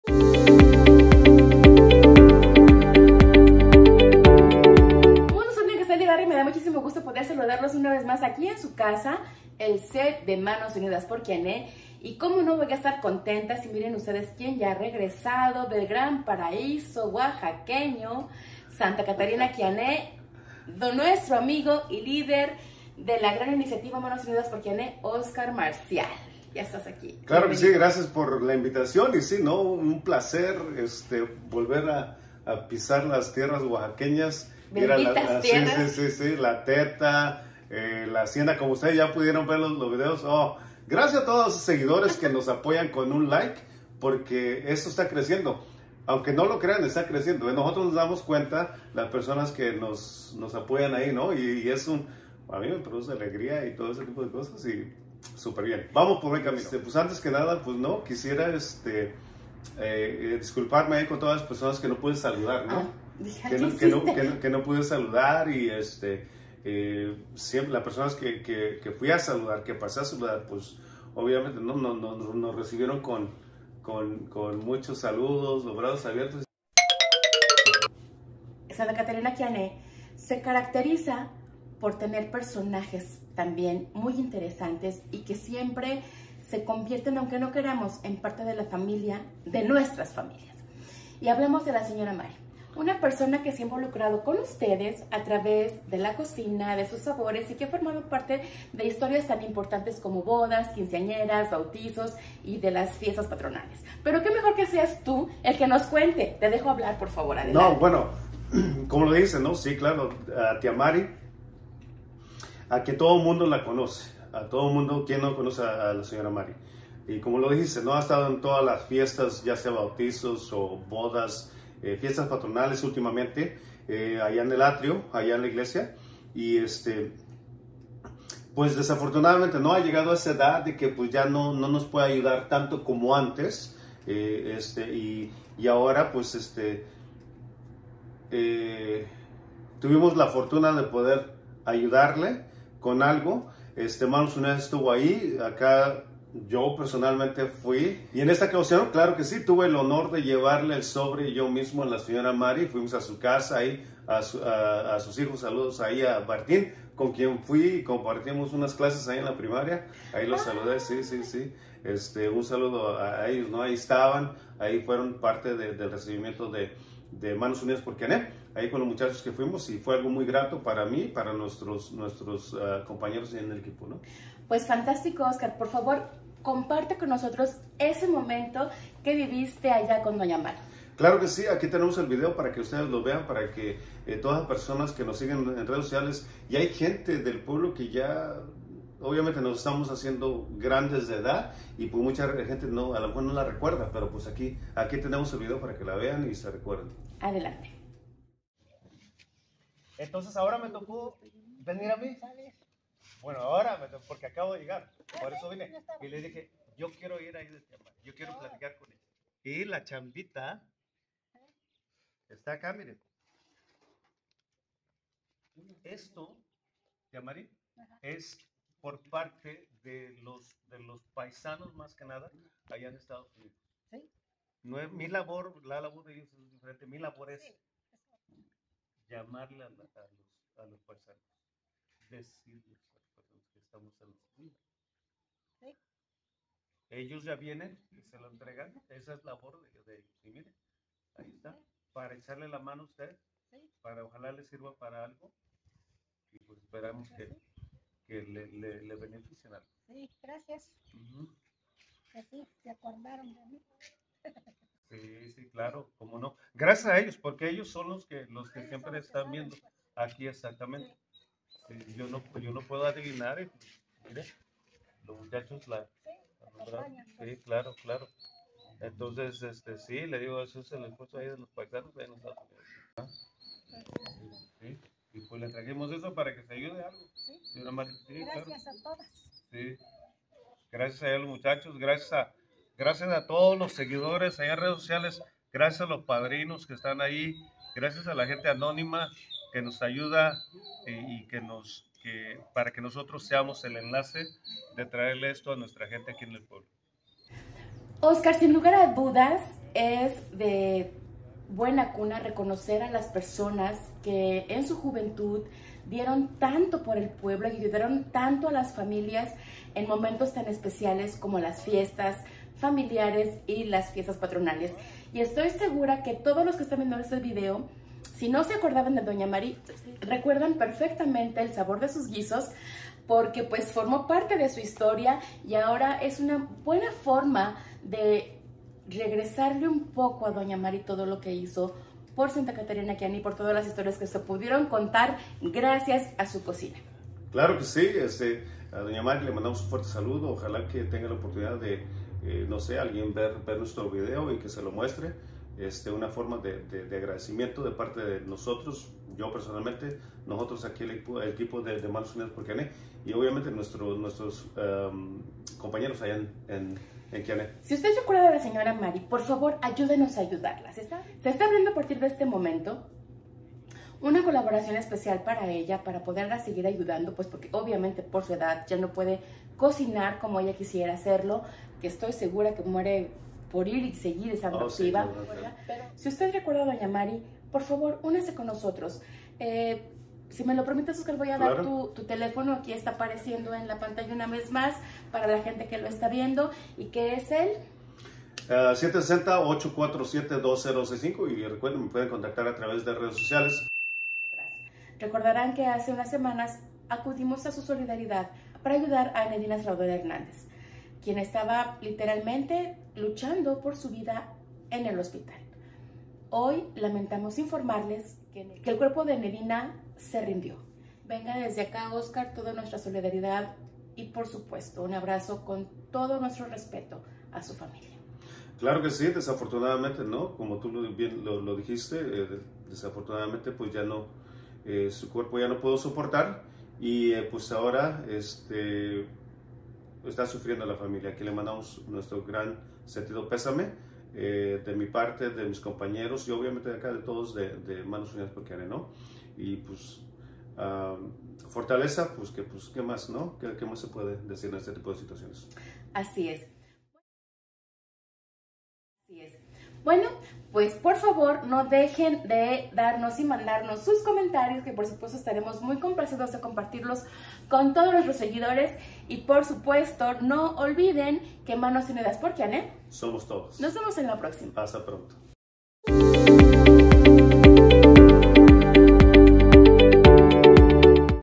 Tia Mary ok HD 720p
Tia Mary siempre formó parte de las historias felices de Quiané a través de su cocina y sus platillos preparados en todas las bodas, bautizos, cumpleaños y no podían las fiestas patronales. Es nuestro turno de devolverle un poquito de todo lo que nos dió. Ayúdanos a ayudarla